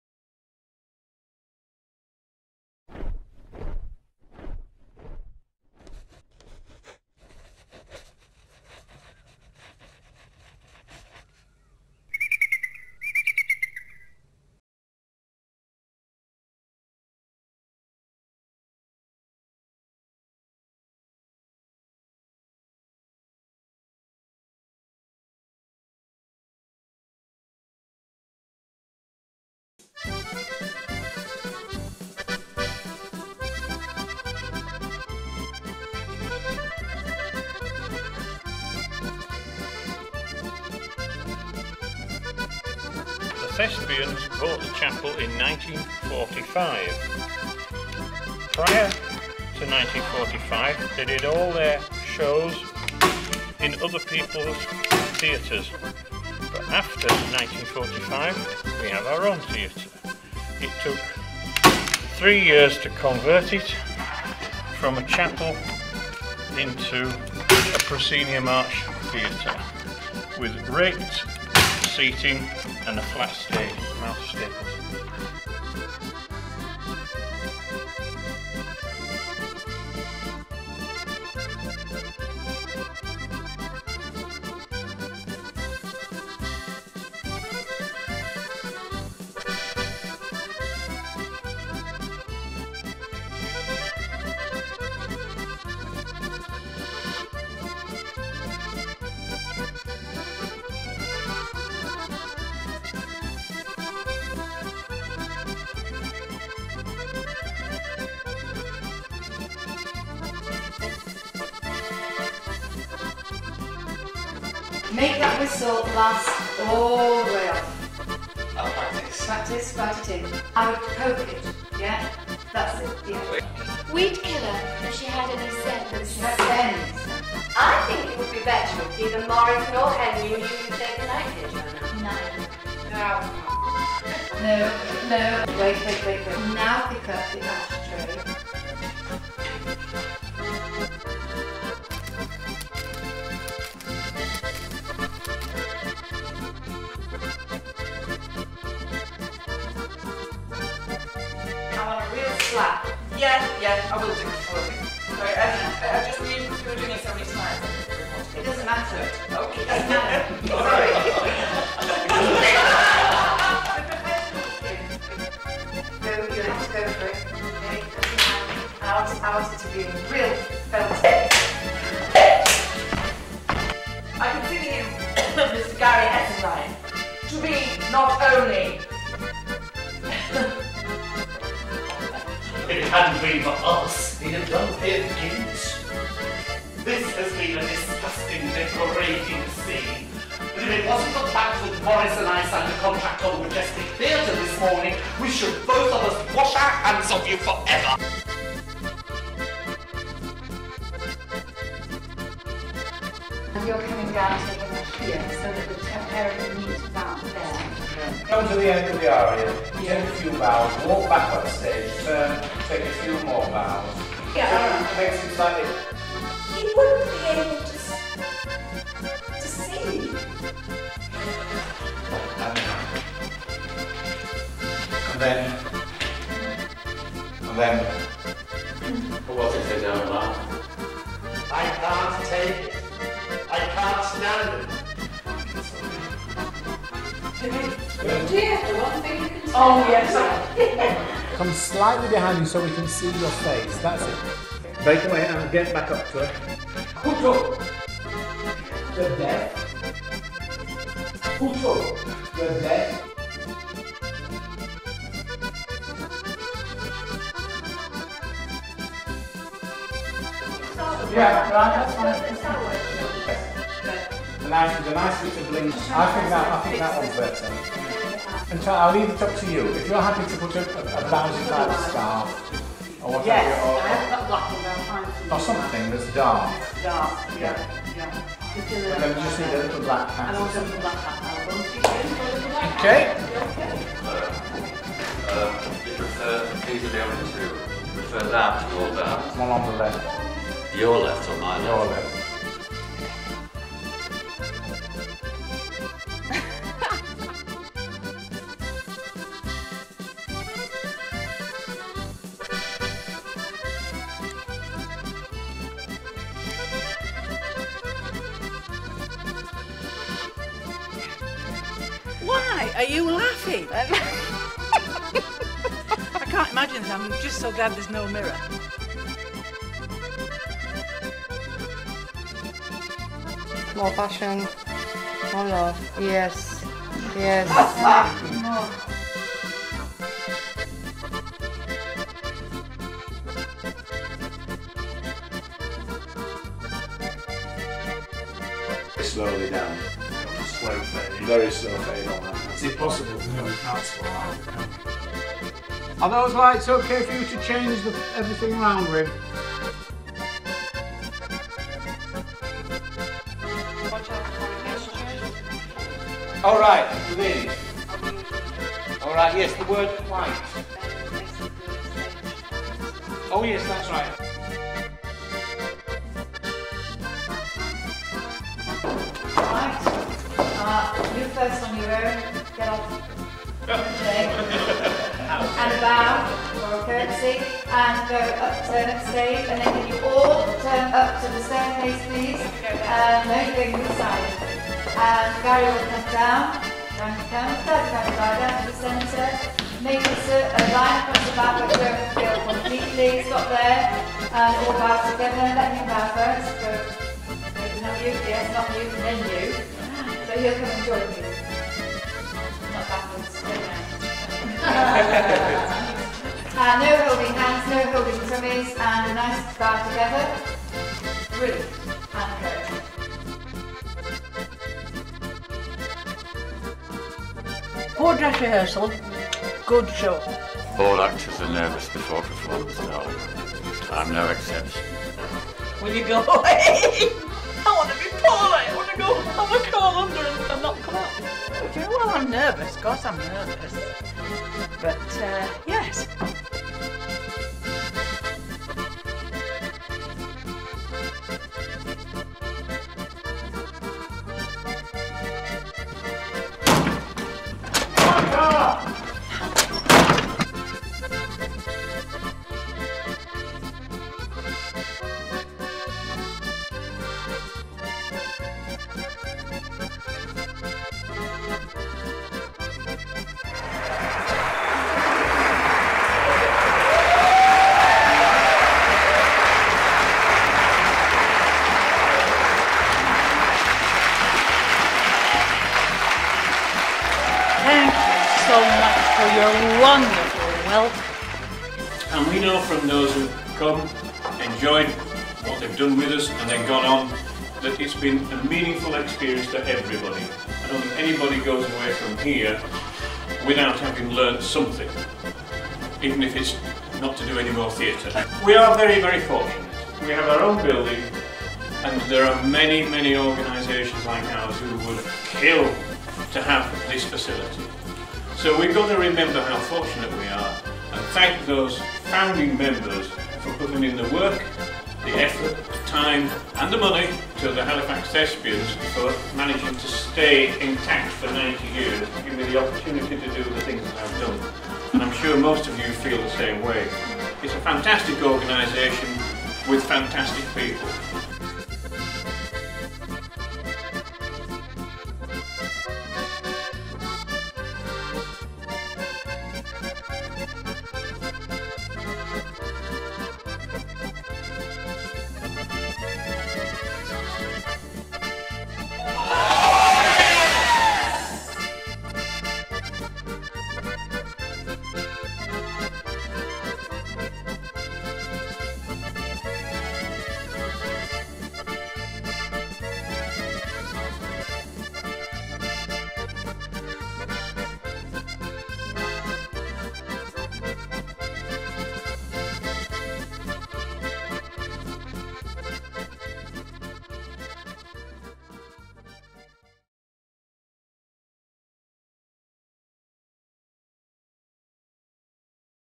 90th Anniversary of Halifax Thespians
An amazing short story about Halifax Playhouse owned and run by Halifax Thespians members based in West Yorkshire.